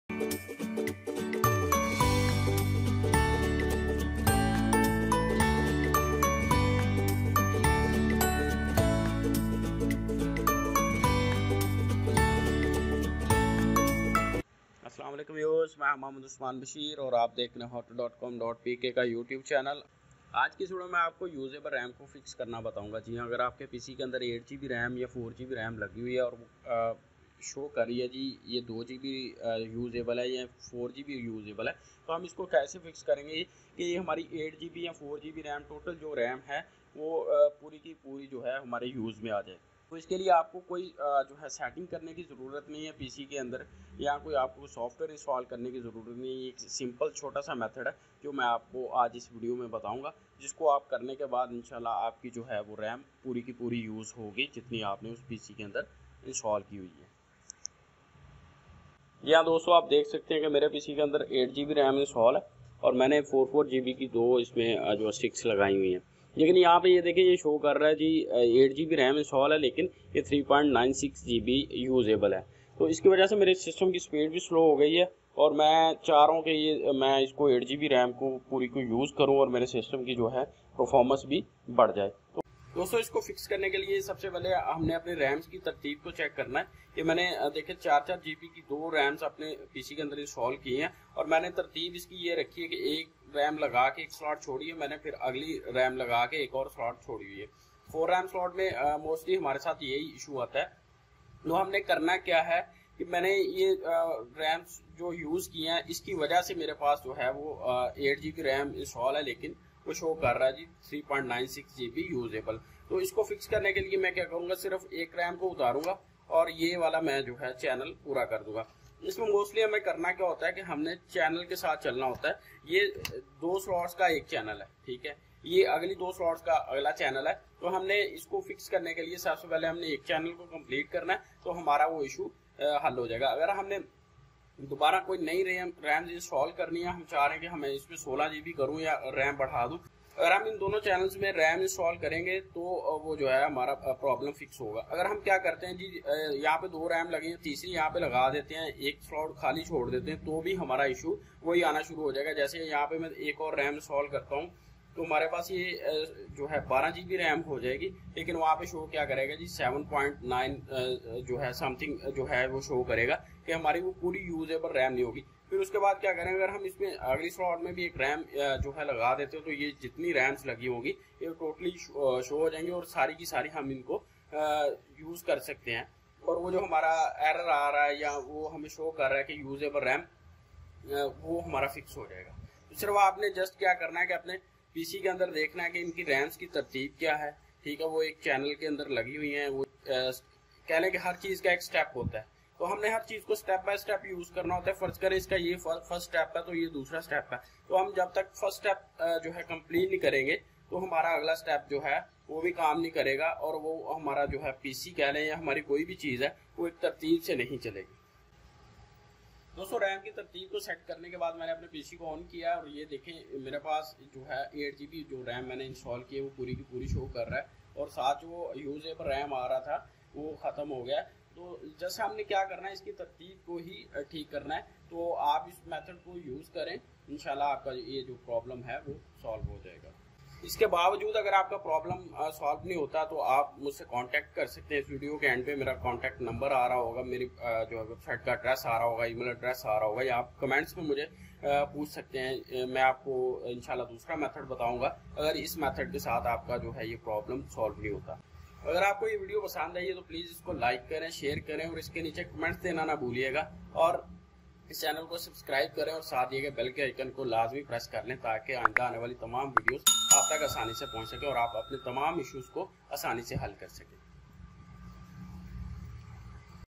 How to fix usable memory may be less than the installed memory | HowTo.com.pk
How to fix usable memory may be less than the installed memory

How do I make Windows 10 use all  RAM? First, you need to update your BIOS. If your PC is still not using all RAM, then change your BIOS settings to make sure Windows 10 isn’t reserving any RAM for secondary GPUs.

Of course, these are only two of the quickest solutions. If the problem persists, here are some additional troubleshooting methods that you can use to fix your RAM.